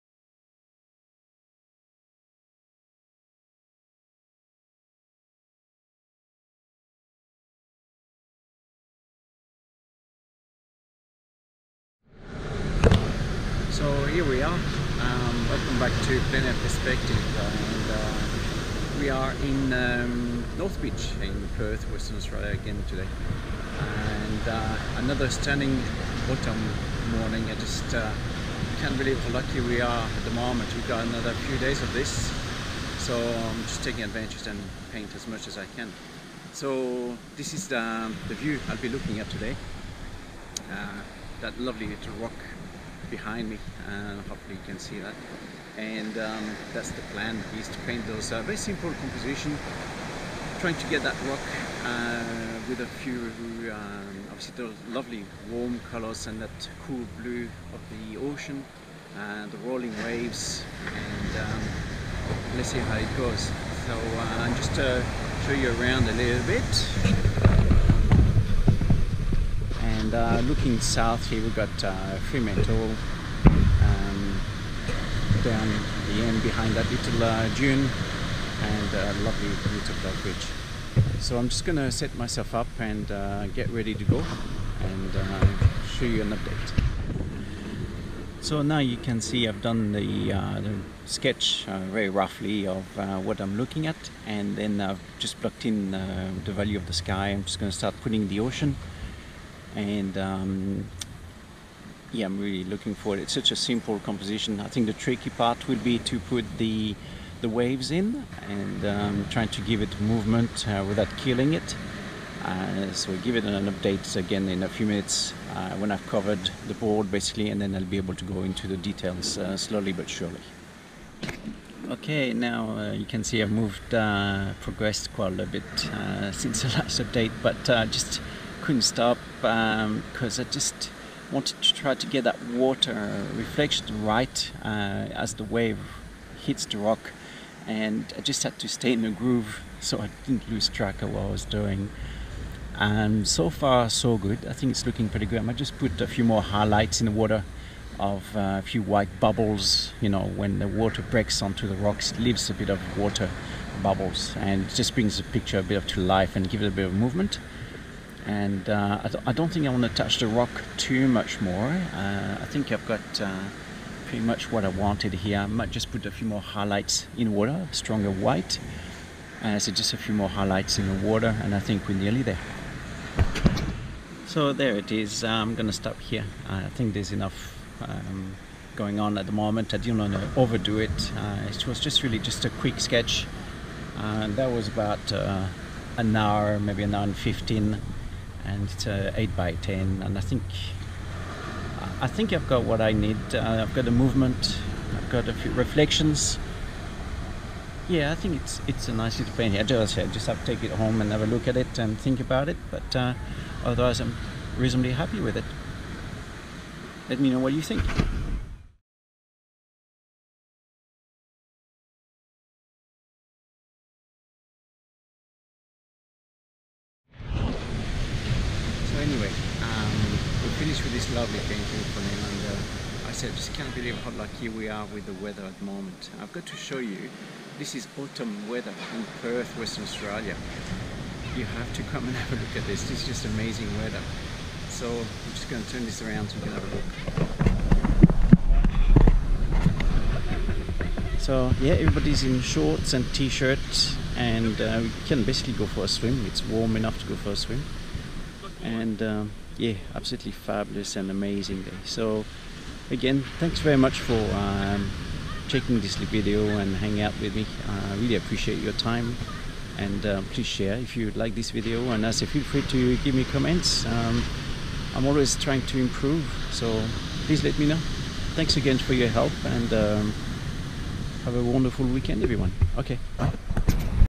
Plein air painting study at North beach, Perth, WA
This video is about a plain air painting in North beach, Perth, WA